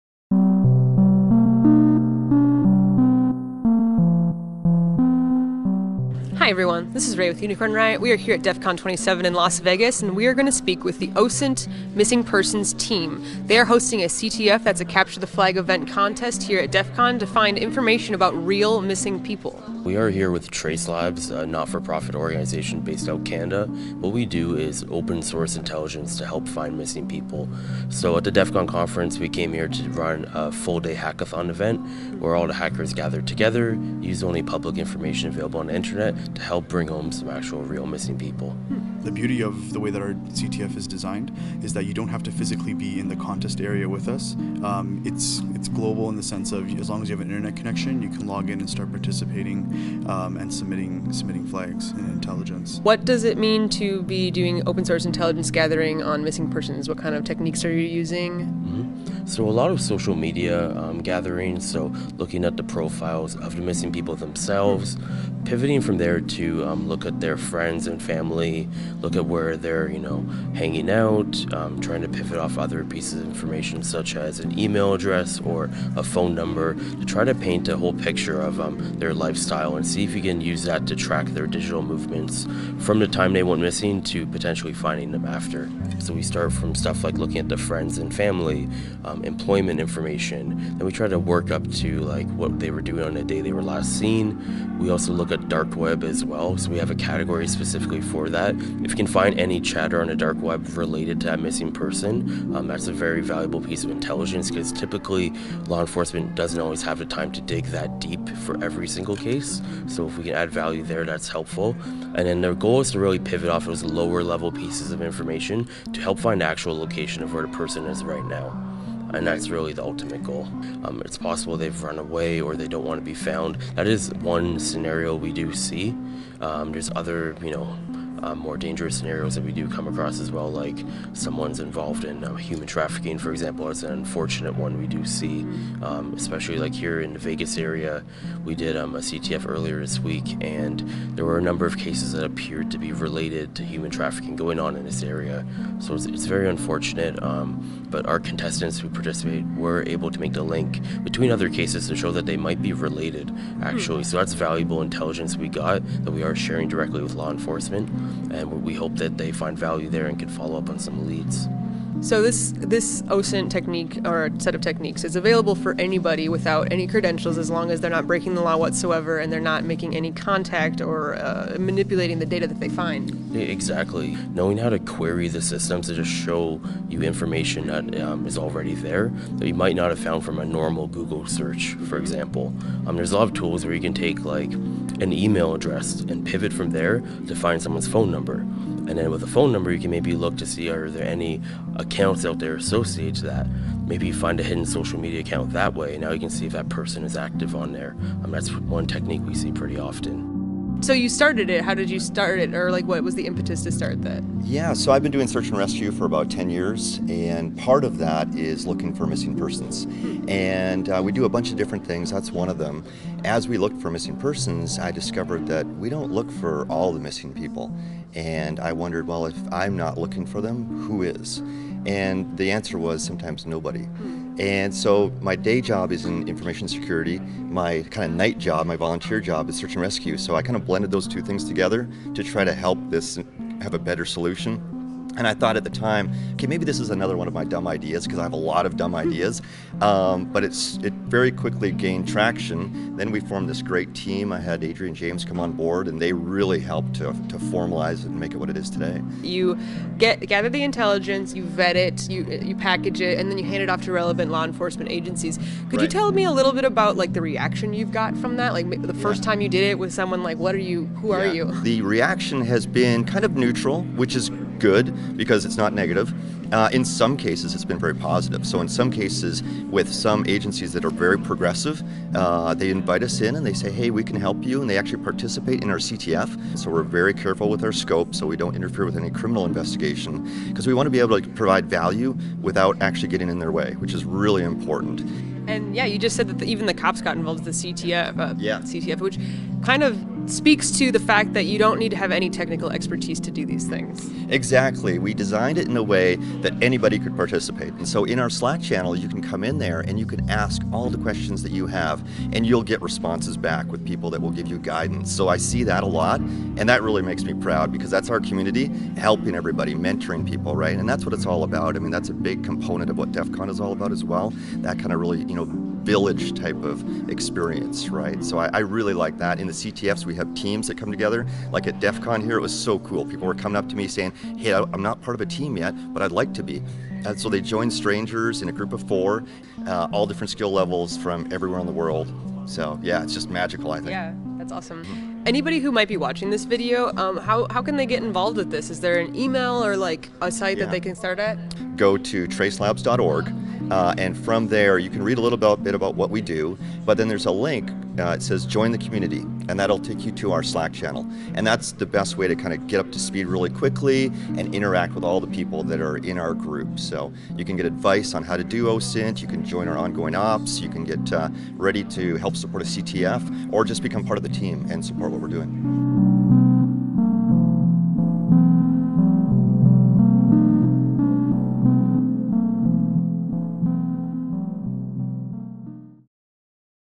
Using OSINT to Find Missing Persons - Interview w/ Trace Labs @ DEFCON 27
At DEFCON 27 in Las Vegas, Unicorn Riot spoke with the organizers of an event where participants were asked to find information on real missing people in a competition called Open-Source Intelligence (OSINT) Missing Persons Capture The Flag (CTF).